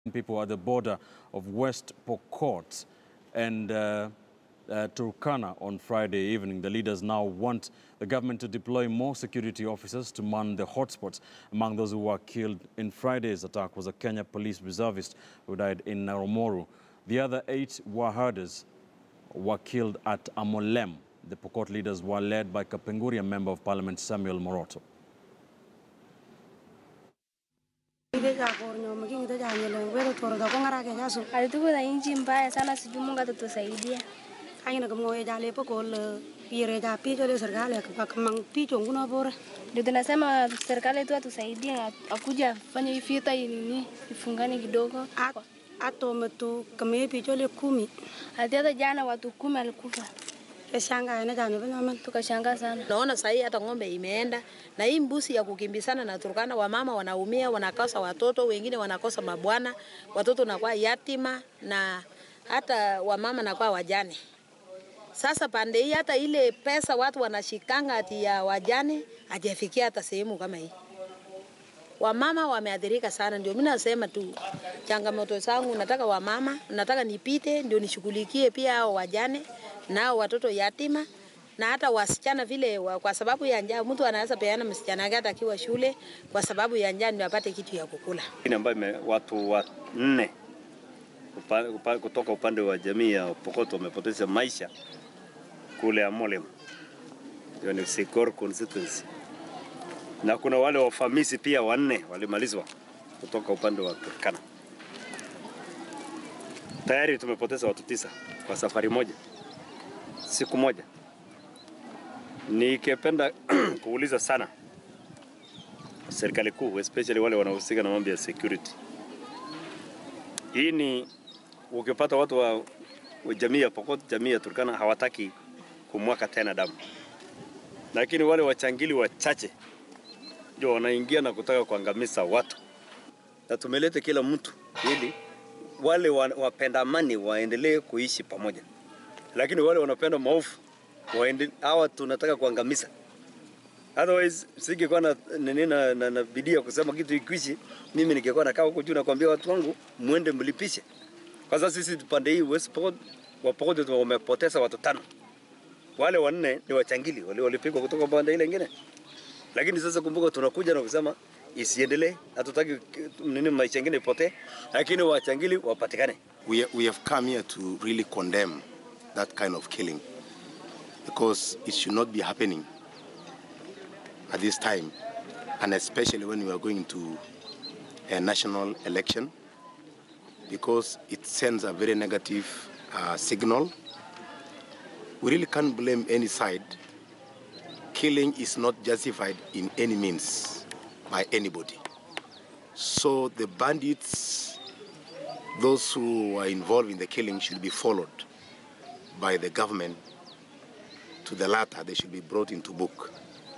Pokot leaders blast government over continued killings in West Pokot
Pokot leaders blast government over continued killings in West Pokot. This comes after nine people were killed on Friday including a police reservist.